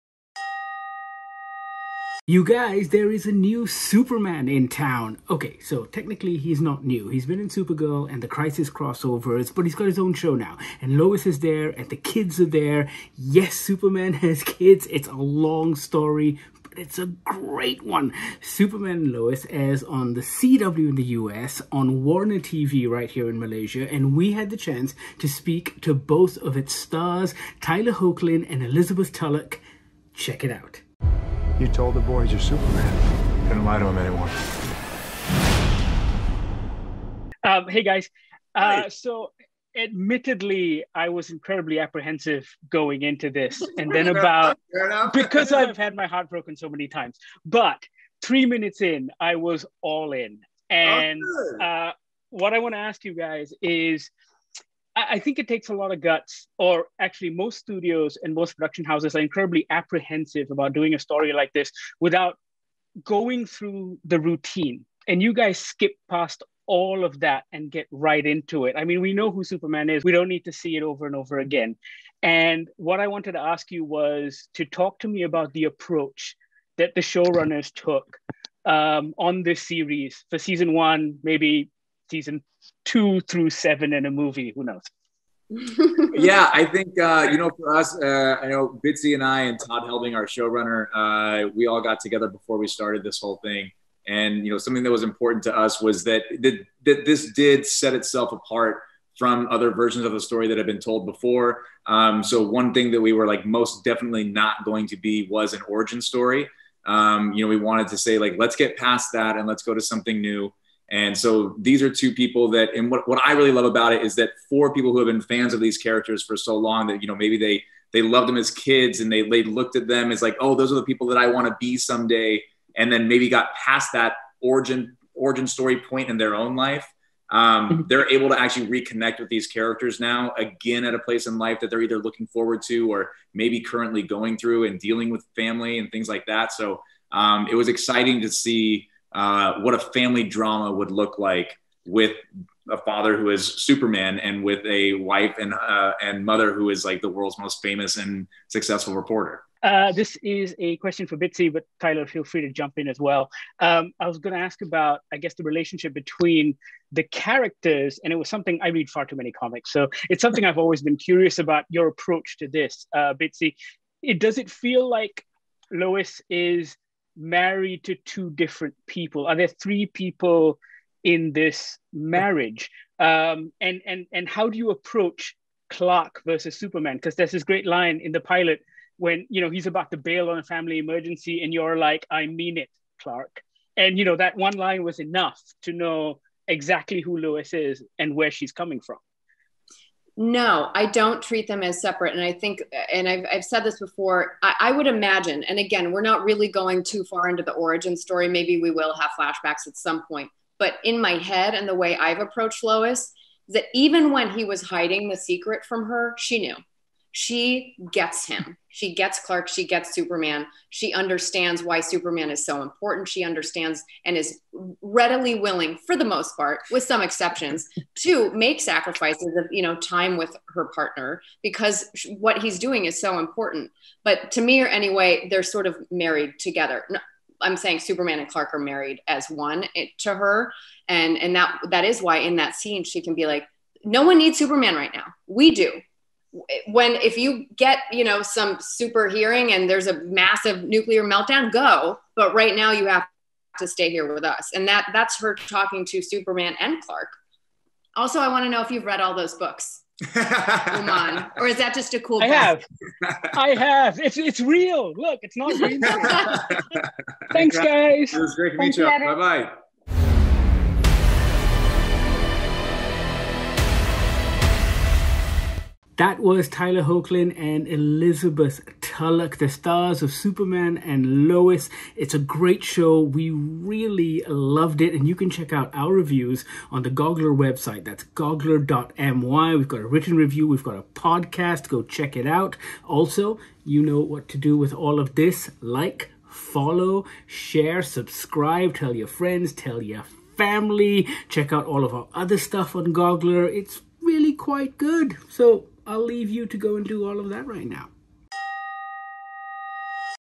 Superman & Lois: A Conversation with Tyler Hoechlin and Elizabeth Tulloch
We spoke to Superman & Lois Lane about their new show and their fresh new approach to this iteration of the comic book power couple.

Head over to Goggler.my for more movie and tv reviews.